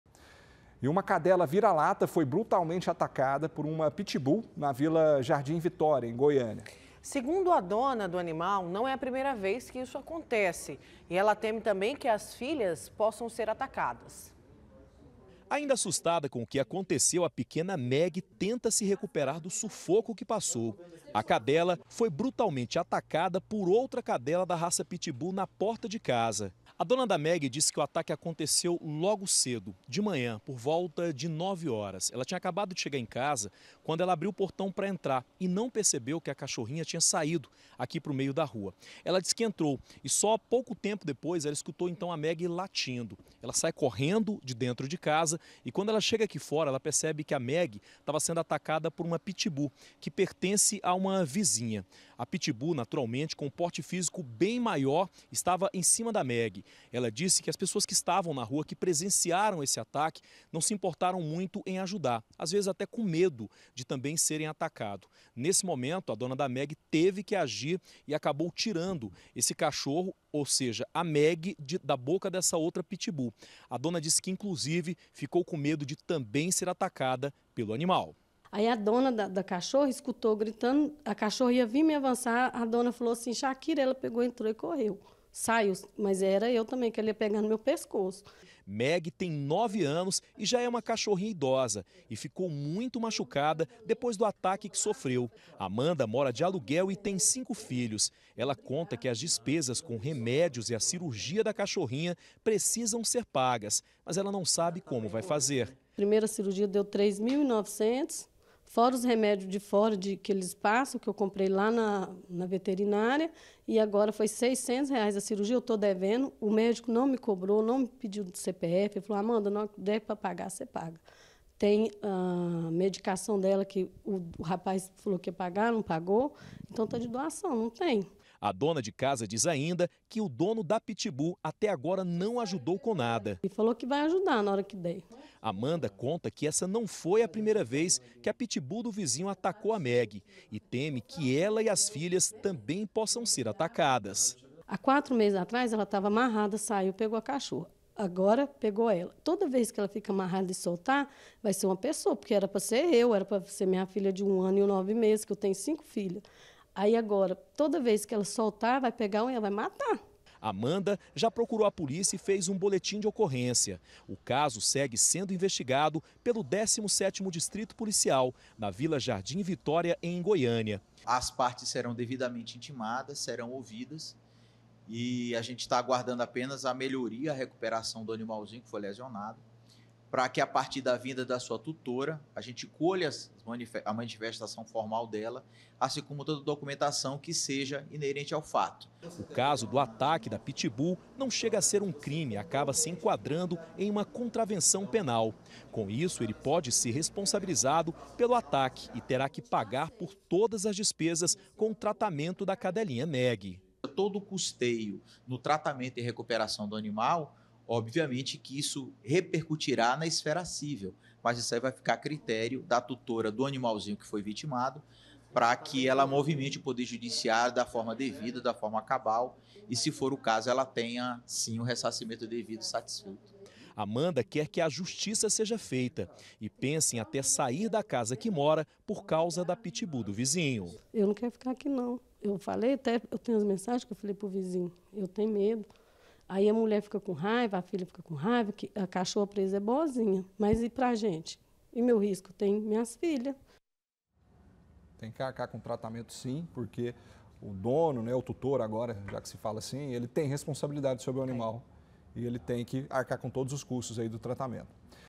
DONA COM MEDO: PIT BULL ATACA CADELA NA RUA
UMA CADELA VIRA-LATA FOI BRUTALMENTE ATACADA POR UMA PIT BULL NA VILA JARDIM VITÓRIA, EM GOIÂNIA. SEGUNDO A DONA DO ANIMAL NÃO É A PRIMEIRA VEZ QUE ISSO ACONTECE. E ELA TEME QUE AS FILHAS DELA TAMBÉM SEJAM ATACADAS.